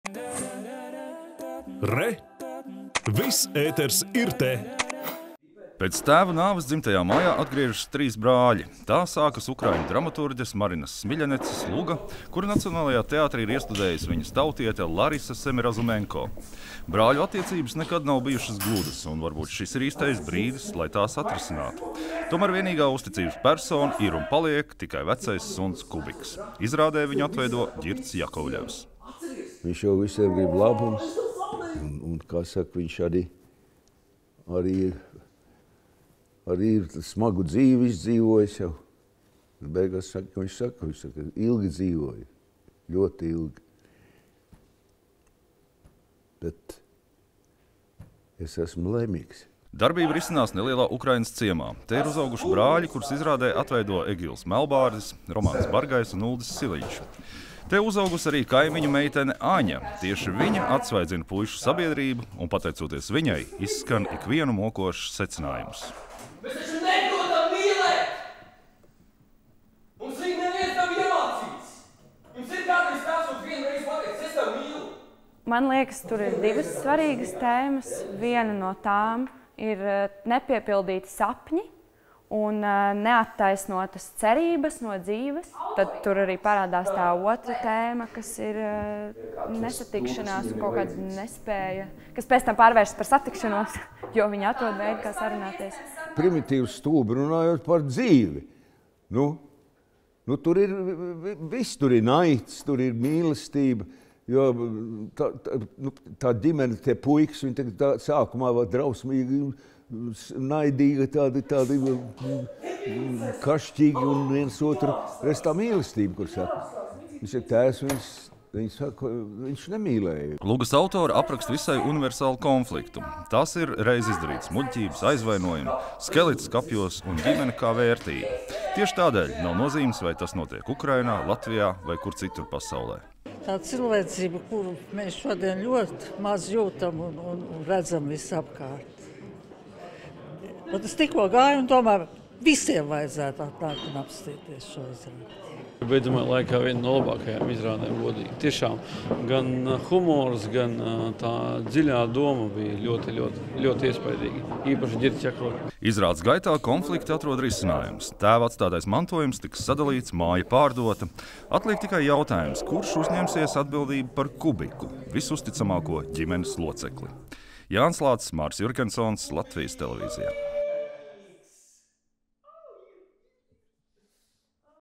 “Suņa māja” Nacionālajā teātrī
Ukraiņu režisore Larisa Semirazumenko Nacionālajā teātrī ir iestudējusi savas tautietes Marinas Smiļanecas drāmu “Suņa māja”. Tā stāsta par trim brāļiem, viņu attiecībām un vecu suni, kas to visu vēro no malas. Kā dzīvot? Kā sarunāties? Kā mīlēt? Tie ir jautājumi, uz kuriem lugas varoņi meklē atbildes. Vairā par iestudējumu stāsta Jānis Lācis.